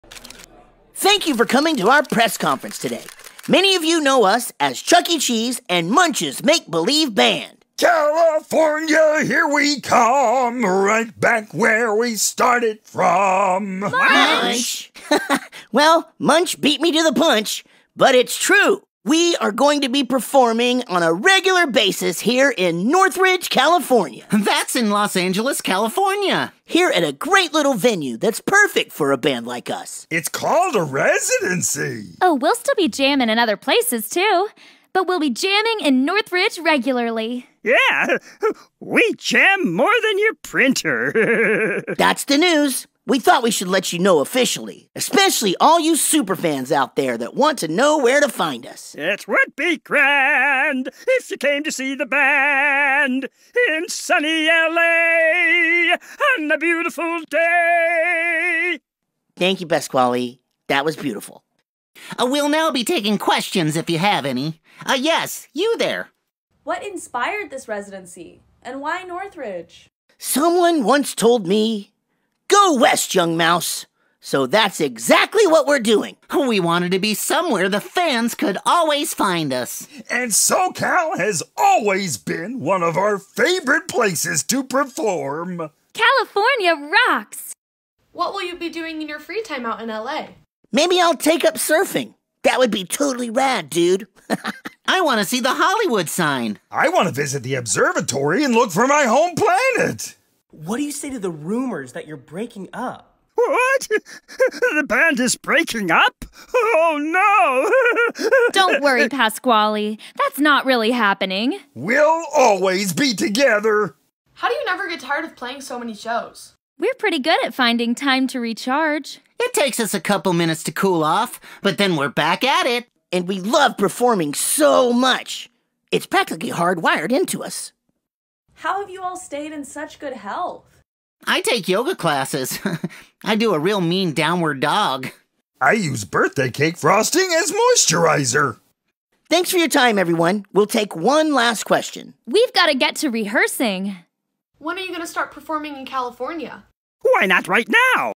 Chuck E. Cheese | BIG ANNOUNCEMENT
Hey guys!!
Guess what?!?
It’s been confirmed.

The animatronics are staying. I said this a lot on my past videos but now it’s confirmed to the public!